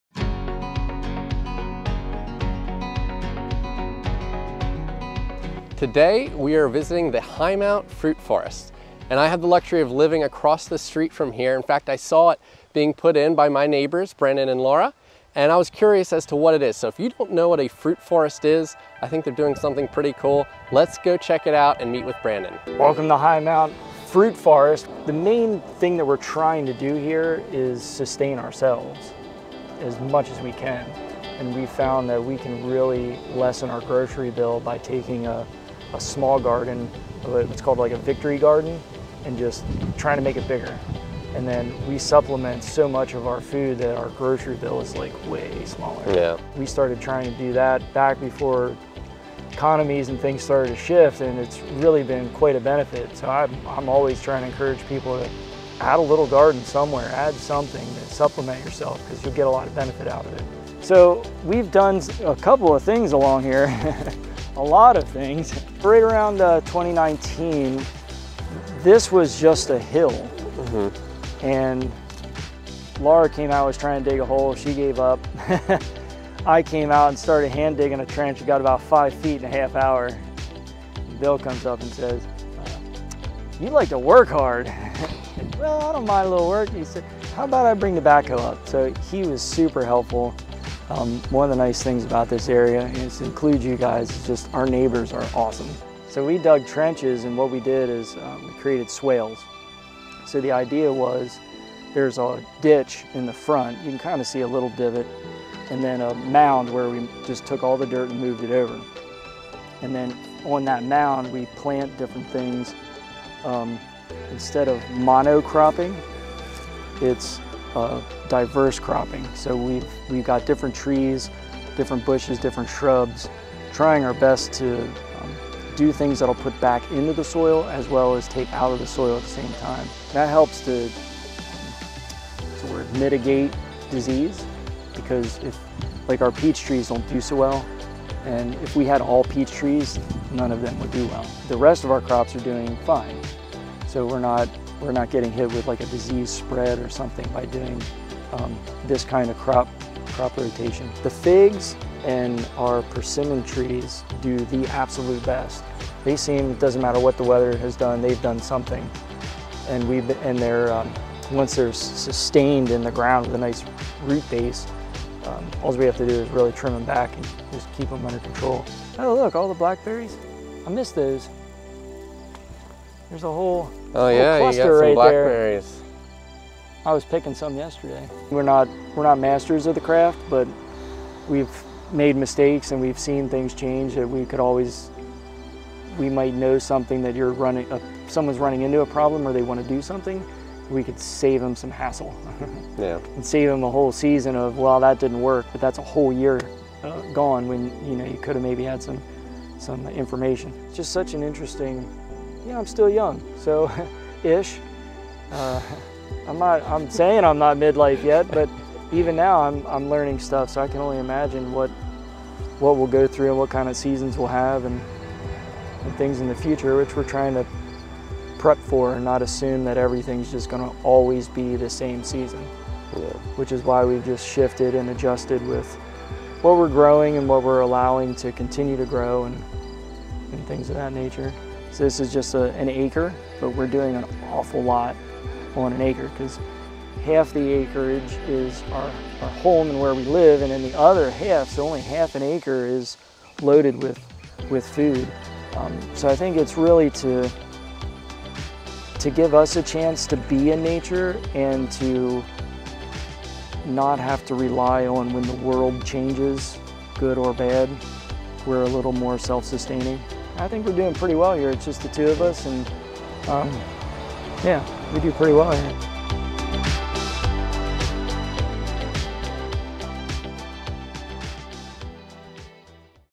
Highmount Fruit Forest
It's a unique place that helps its owners live a more self-sustainable life, and could inspire you to do the same!